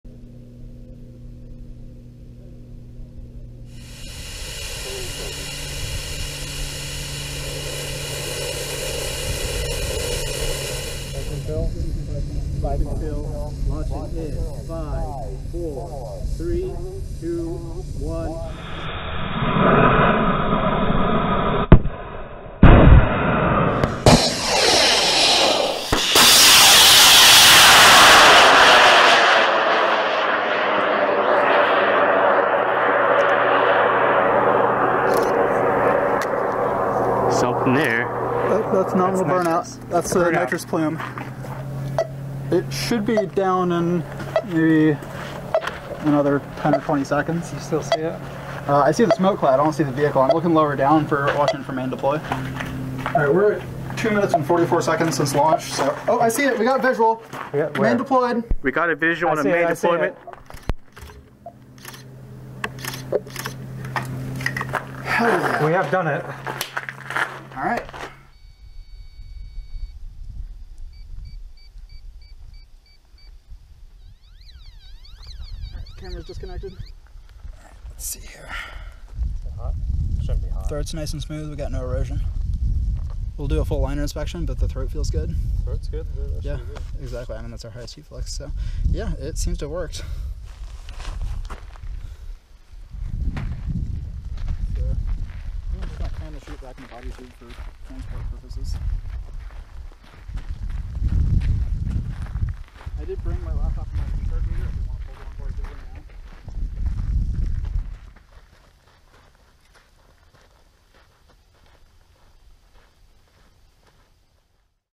3CAT/8 Takes Flight (Half Cat Rocketry)
Half Cat Rocketry brings out a new rocket, 3Cat/8, made from high power rocket components and equipped with a new aluminum regenerative pressure fed Nitrous IPA motor. In it's first ever flight, it flew up to 8750 feet and recovered nominally.